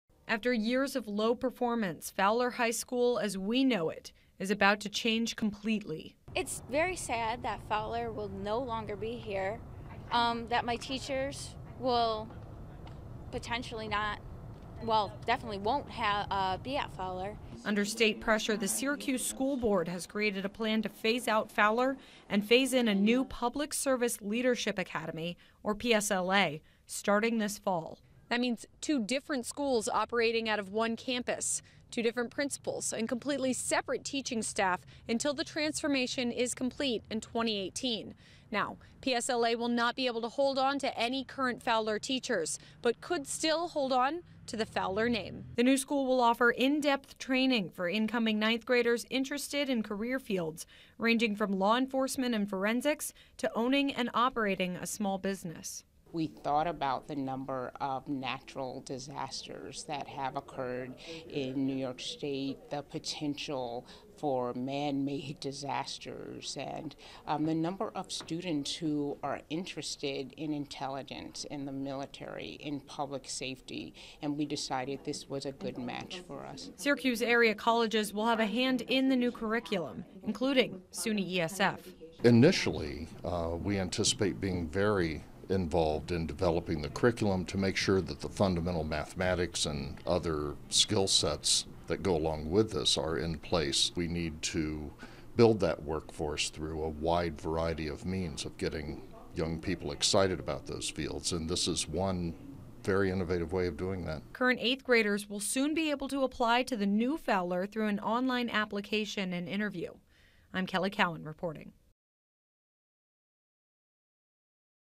New technical school to replace Fowler High School
After years of low performance, the state has stepped in and asked the Syracuse City School District to implement sweeping changes at three area schools. Tonight at a special meeting the SCSD Board voted to phase out Fowler High School and open a new Public Service Leadership Academy in the same location.